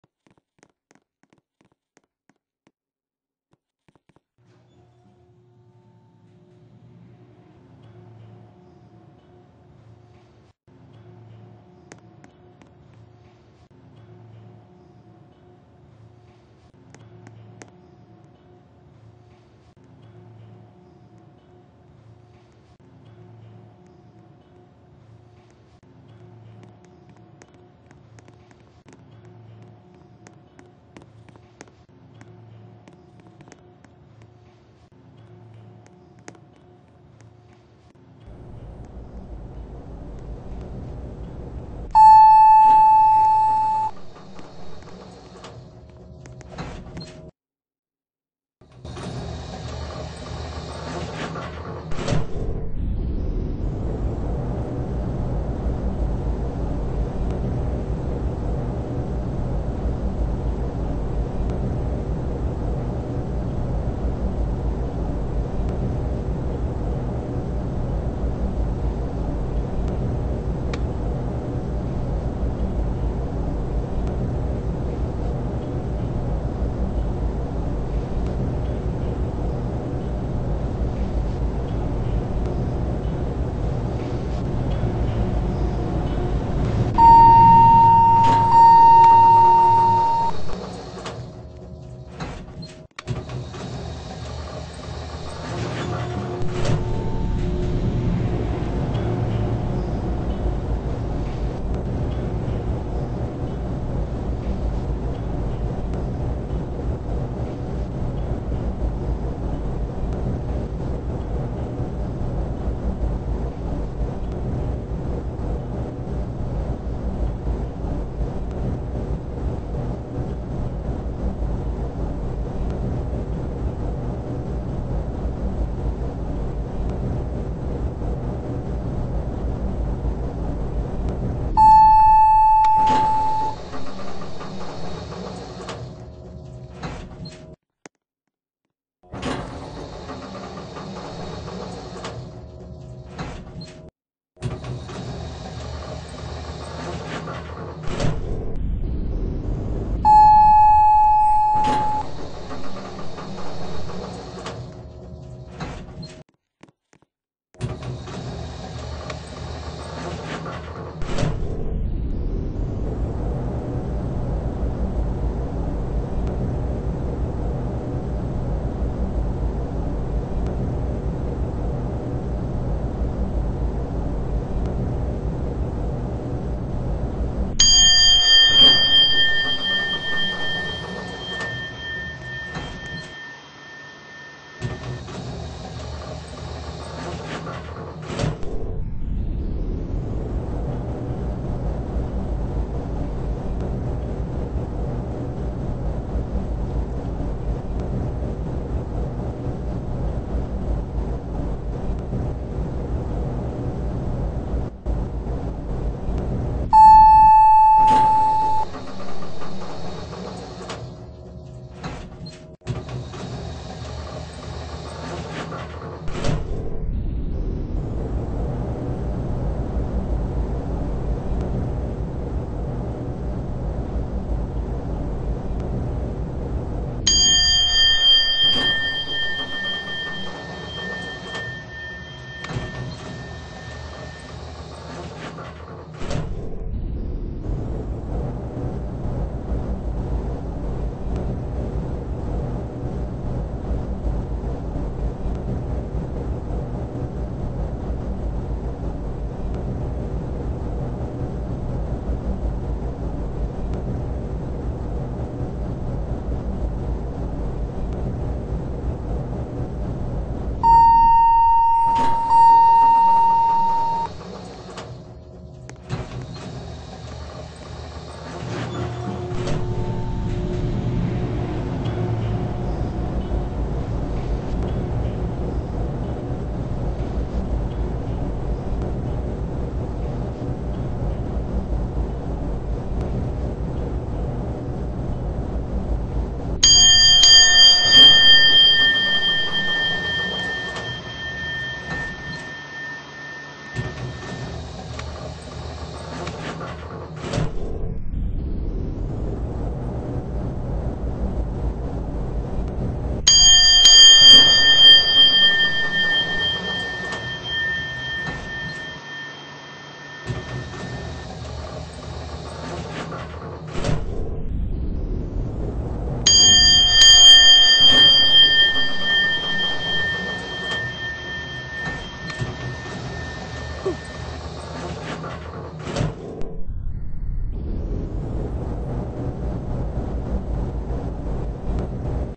(1-26-2020) Westinghouse Low-rise elevators @ Grand Four Points Hotel
This was filmed after the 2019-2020 expansion,  also was filmed before the upgrades and renovations to the elevators.
ELEVATOR BRAND
   Installer: Westinghouse
YEAR INSTALLED: 1962
YEAR UPGRADED: N/A
FIXTURES (ORIGINAL)
   Call Stations: Westinghouse AE
   COP: Westinghouse AE
DOORS
   Type: Center-opening
MACHINE
   Drive: Traction
CAPACITY
   Pounds: N/A
   Persons: N/A
CHIMES: Electronic (LL, AL, 3, 36) Dinner Bell (4-19)
INDICATOR: Westinghouse analog indicator
PHONE: N/A
FLOORS SERVED: 1-19 and 36
# OF ELEVATORS: 1